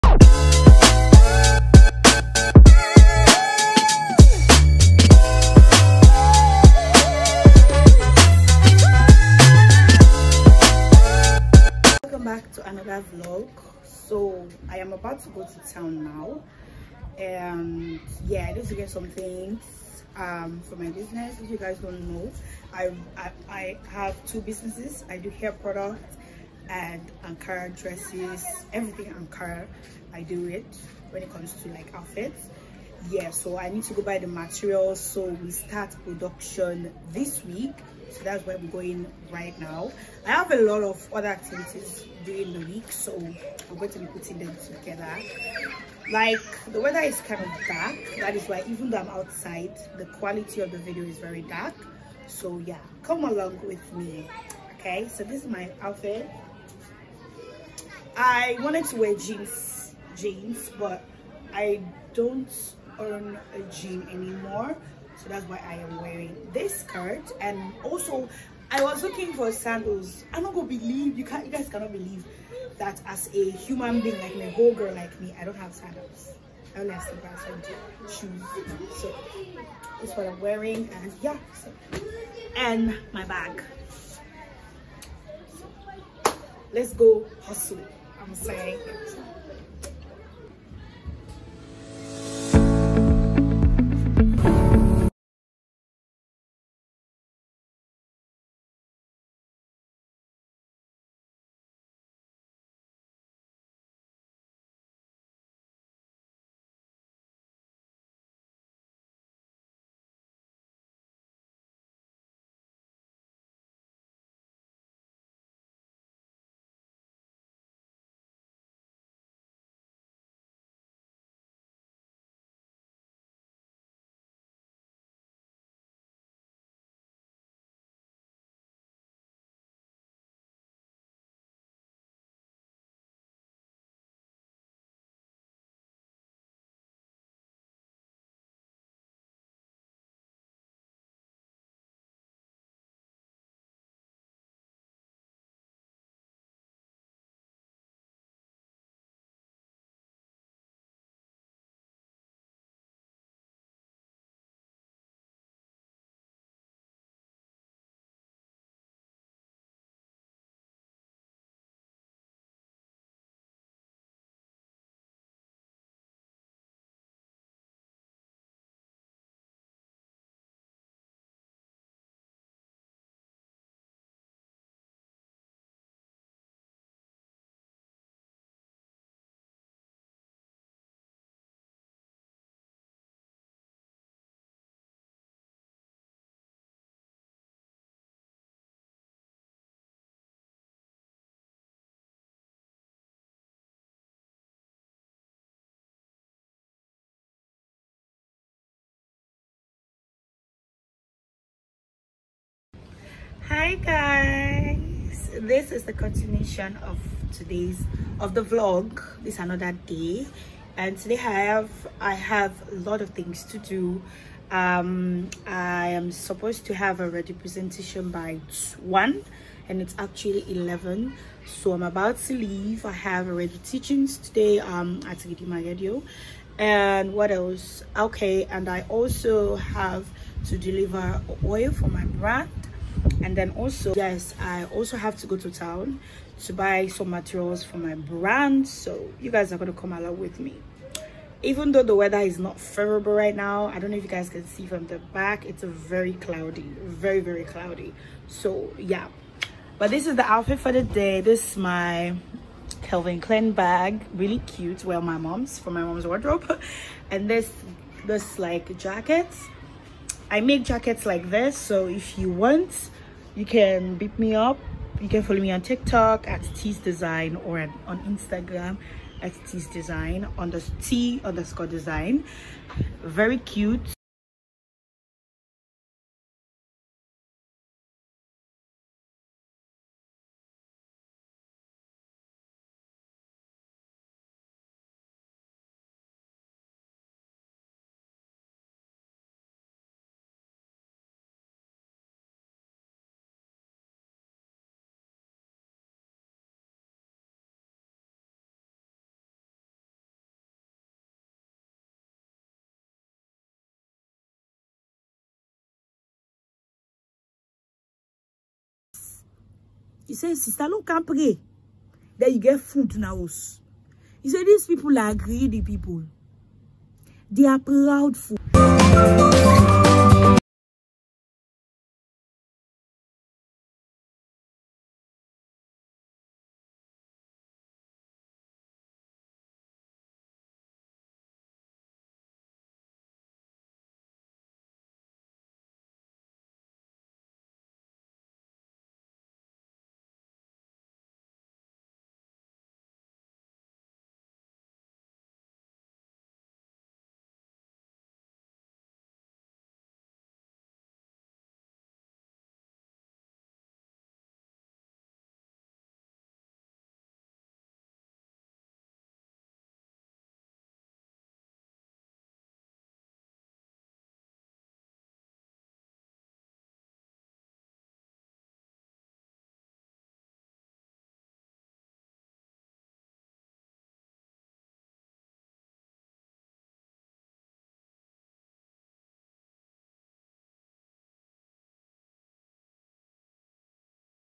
DAILY DIARY: Living in Sierra Leone🇸🇱,starting a business,preparing for Law school#viralvideo
Hey guys